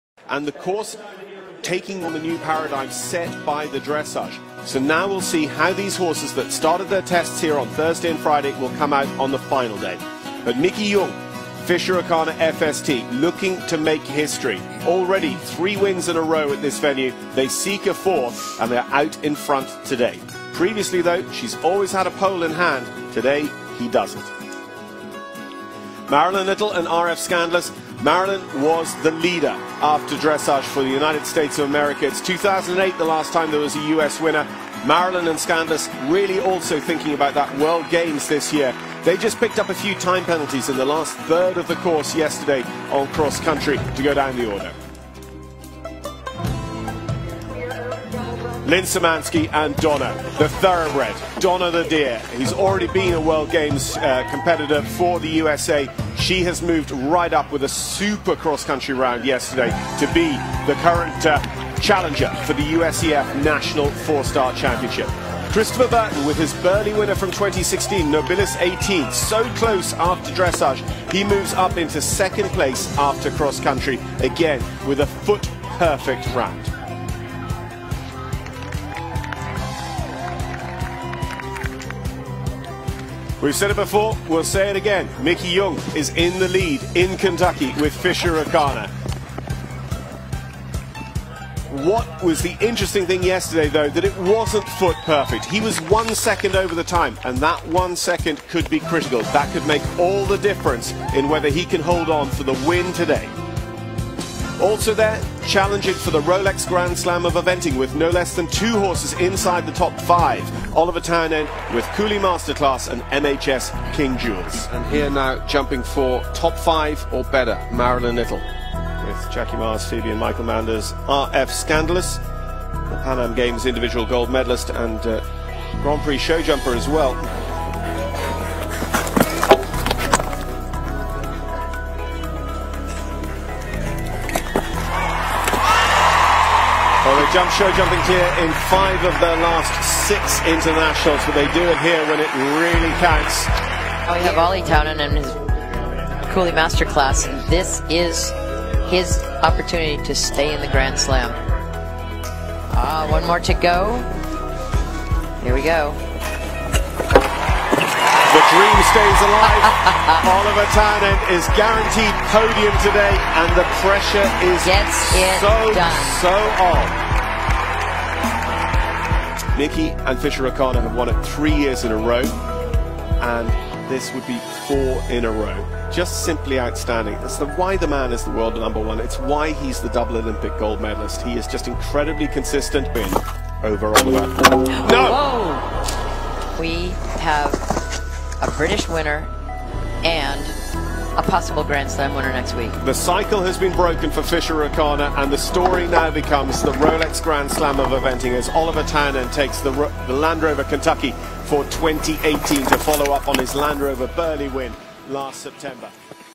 Best of 2018: Land Rover Kentucky Three Day Event
As the year comes to a close we're looking back on some of our favorite moments from 2018 starting with highlights from the Land Rover Kentucky Three-Day Event aka the BestWeekendAllYear!